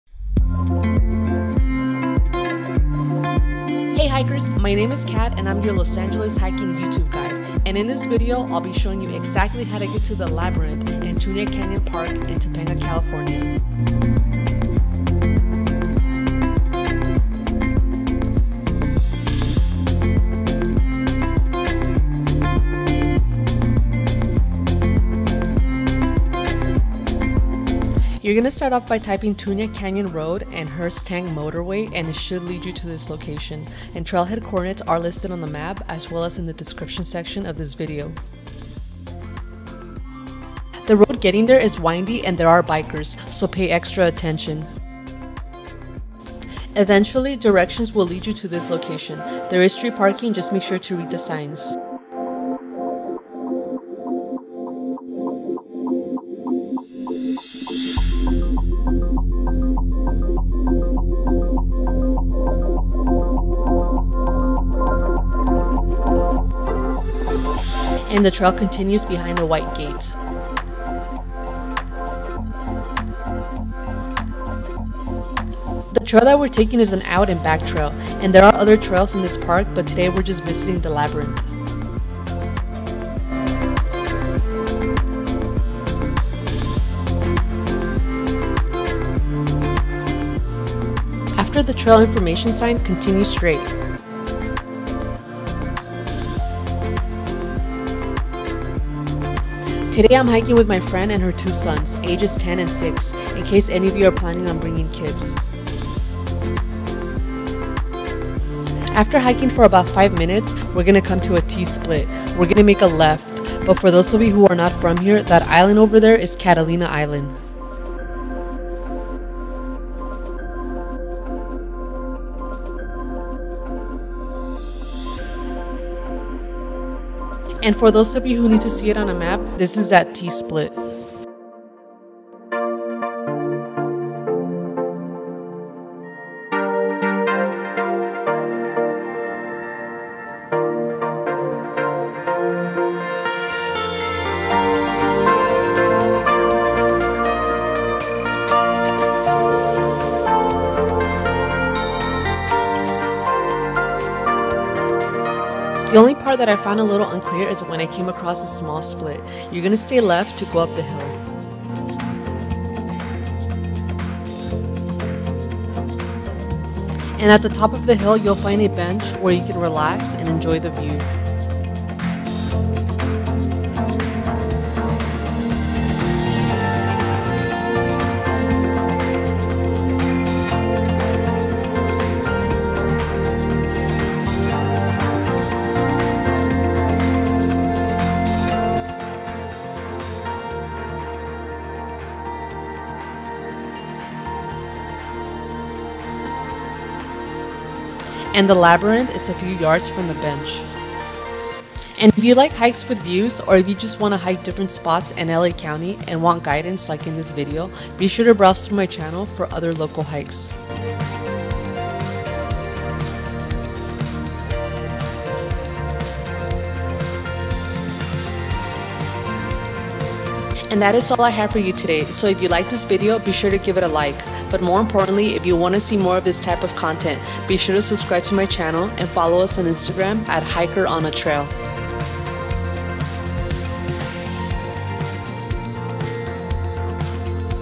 Hiking Labyrinth, Tuna Canyon Park, Coordinates 34.05956,-118.6165 Trail Guide, Directions
**TRAILHEAD COORDINATES**
34.05956,-118.6165
-Hey Hikers, totally listed wrong Trailhead Coordinates on video. These are the correct ones, happy Hiking :)

This video guides you to the Labyrinth at Tuna Canyon Park in Topanga, CA. Exact Directions.

STATS
Time: 40 Minutes Round Trip
Total Distance: 1.5 Miles (Out and Back)
Elevation: 267 Ft. (Per my STRAVA)
Rated: Easy

TRAILHEADS DIRECTIONS
-Type: Tuna Canyon Rd. & Hearst Tank Mtwy.
**TRAILHEAD COORDINATES**
34.05956,-118.6165
-The hike starts behind white gate

ADDITIONAL INFO:
*There are NO porta potty or water fountains at the trailhead so plan accordingly.
*There is Street Parking on Tuna Canyon Rd. read Parking Signs
*I am only hiking to the Labyrinth. Park offers other hikes. Explore!